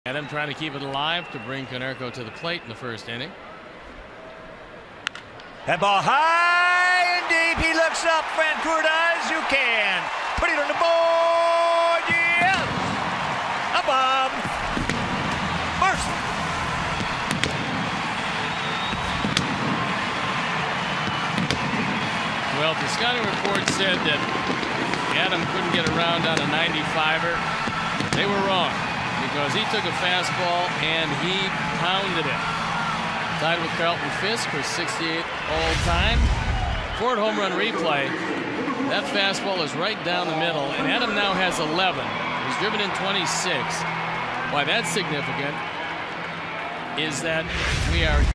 2012/05/11 Dunn's solo homer
KC@CWS: Dunn jacks his 11th homer of the season